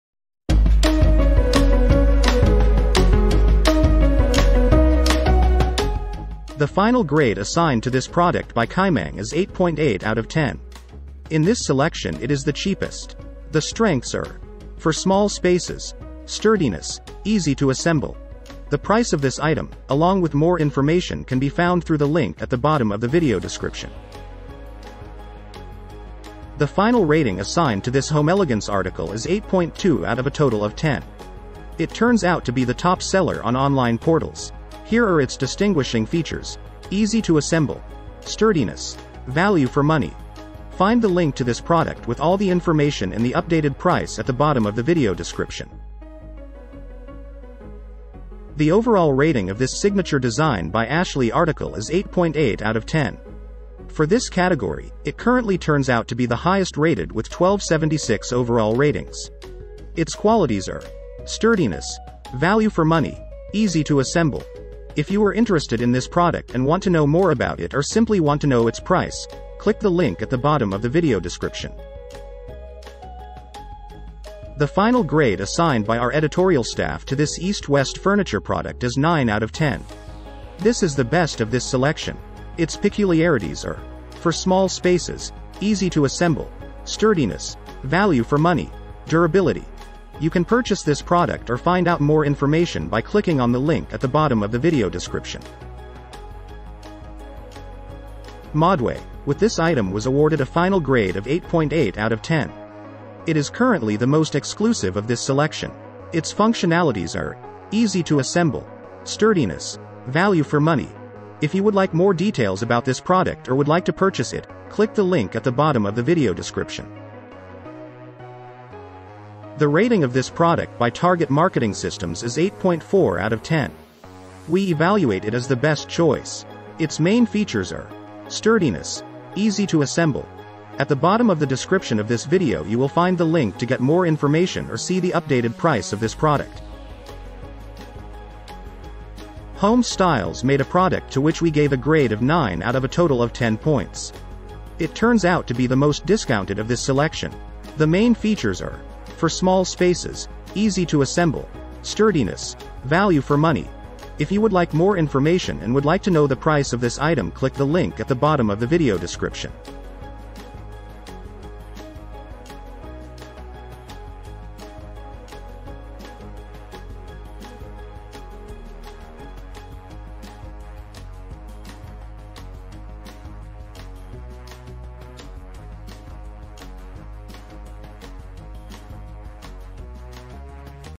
Round dining tables - Top 7 - (ChooseProducts)
📣 'Round dining tables' links featured in this video:

✅ THE CHEAPEST: KaiMeng White Round Dining Table 32” Modern Pedestal Small C...

✅ THE BEST-SELLING: Homelegance Fideo 45” Round Industrial Style Dining Table, P...

✅ THE MOST VOTED: Signature Design by Ashley Glambrey Old World 45” Round Glas...

✅ THE BEST: East West Furniture Wooden DLT-MAH-TP Dining Room Table Roun...

✅ THE MOST EXCLUSIVE: Modway Lippa 40” Mid-Century Dining Table with Round Artific...

✅ OUR CHOICE: Target Marketing Systems 44201 Round Drop Leaf Table with Tu...

✅ THE MOST OBVIOUS: French Countryside Oak/ White 42” Round Pedestal Table by Ho...

Index:
00:00 - Intro
00:05 - KaiMeng White Round Dining Table 32” Modern Pedestal Small C...
00:25 - Homelegance Fideo 45” Round Industrial Style Dining Table, P...
00:47 - Signature Design by Ashley Glambrey Old World 45” Round Glas...
01:13 - East West Furniture Wooden DLT-MAH-TP Dining Room Table Roun...
01:37 - Modway Lippa 40” Mid-Century Dining Table with Round Artific...
01:58 - Target Marketing Systems 44201 Round Drop Leaf Table with Tu...
02:18 - French Countryside Oak/ White 42” Round Pedestal Table by Ho...